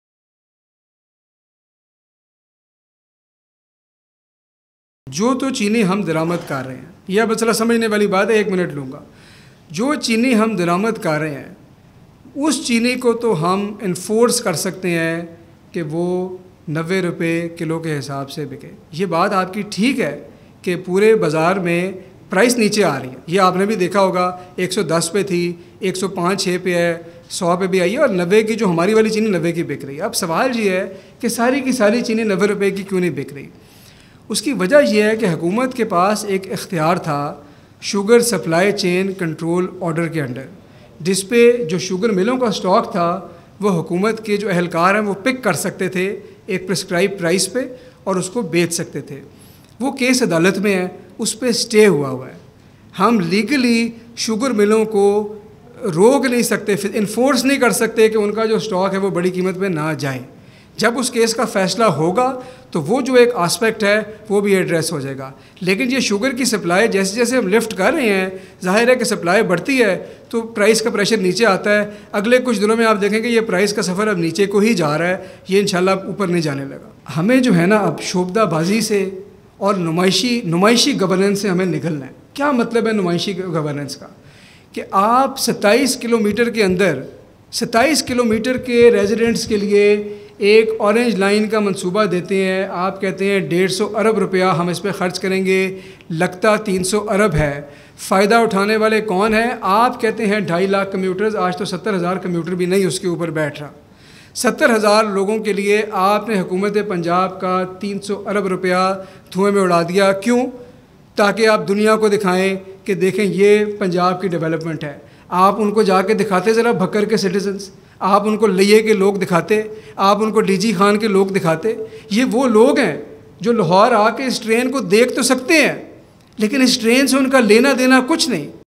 SACM Punjab on Information & Special Initiatives Hasaan Khawar press conference
LAHORE, OCT 15 (APP): SACM Punjab on Information & Special Initiatives Hasaan Khawar press conference.